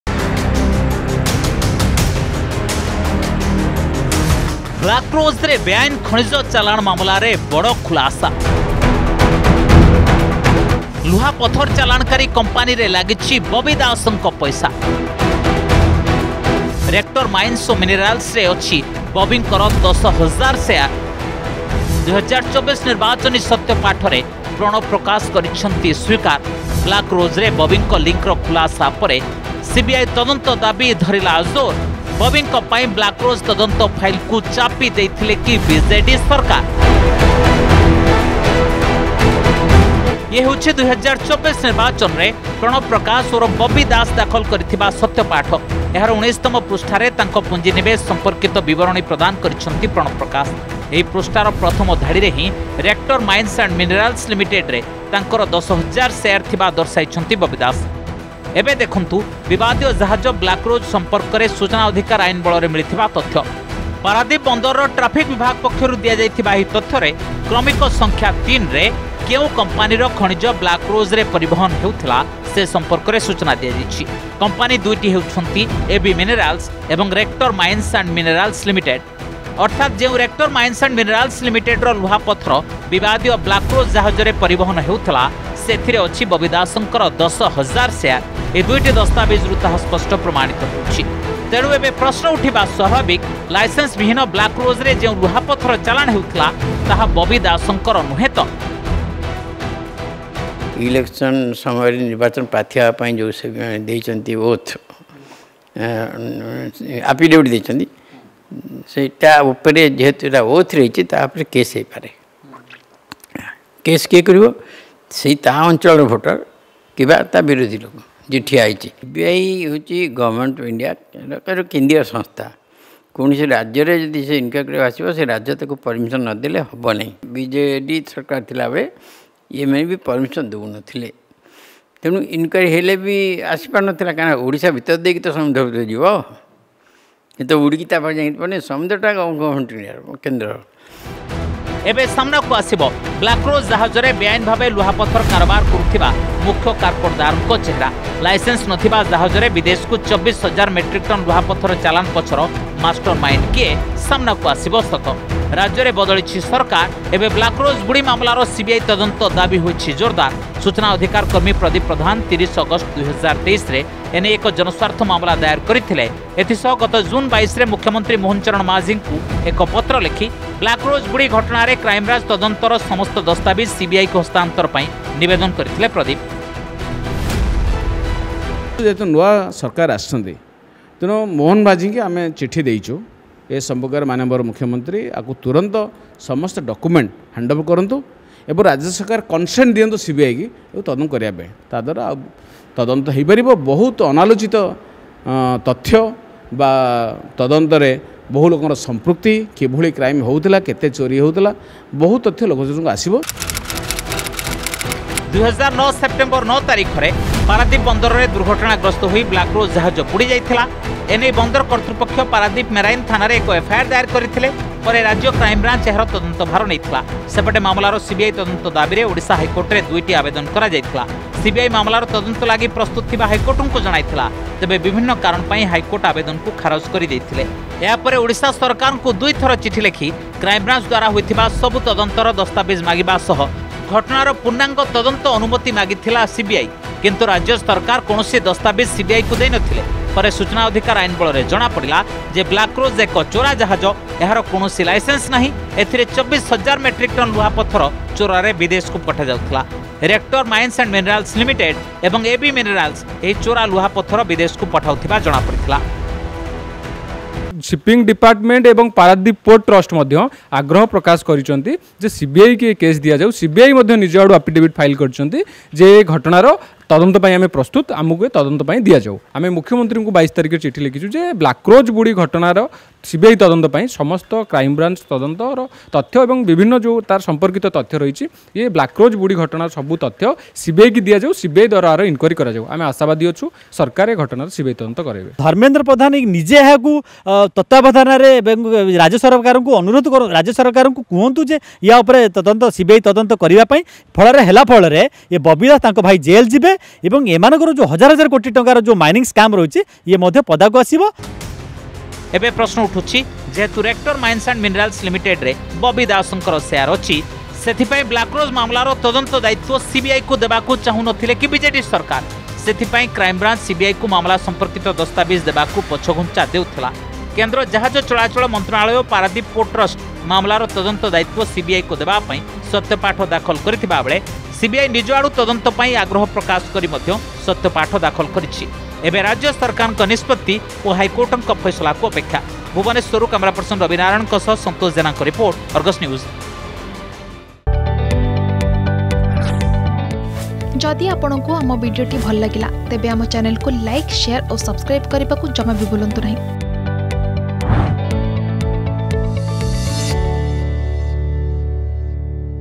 ବ୍ଲାକ୍ ରୋଜରେ ବେଆଇନ୍ ଖଣିଜ ଚାଲାଣ ମାମଲାରେ ବଡ଼ ଖୁଲାସା || Black Rose Illegal Mining Case
ବ୍ଲାକ୍ ରୋଜରେ ବେଆଇନ୍ ଖଣିଜ ଚାଲାଣ ମାମଲାରେ ବଡ଼ ଖୁଲାସା । ଲୁହାପଥରେ ଚାଲାଣକାରୀ କମ୍ପାନୀରେ ଲାଗିଛି ବବି ଦାସଙ୍କ ପଇସା । ରେକ୍ଟର ମାଇନ୍ସ ଓ ମିନେରାଲ୍ସରେ ଅଛି ବବିଙ୍କର ୧୦ ହଜାର ସେୟାର । ୨୦୨୪ ନିର୍ବାଚନୀ ସତ୍ୟପାଠରେ ପ୍ରଣବ ପ୍ରକାଶ କରିଛନ୍ତି ସ୍ବୀକାର । ବ୍ଲାକରୋଜରେ ବବିଙ୍କ ଲିଙ୍କର ଖୁଲାସା ପରେ ସିବିଆଇ ତଦନ୍ତ ଦାବି ଧରିଲା ଜୋର । ବବିଙ୍କ ପାଇଁ ବ୍ଲାକ୍ ରୋଜ୍ ତଦନ୍ତ ଫାଇଲକୁ ଚାପି ଦେଇଥିଲେ କି ବିଜେଡି ସରକାର ? ।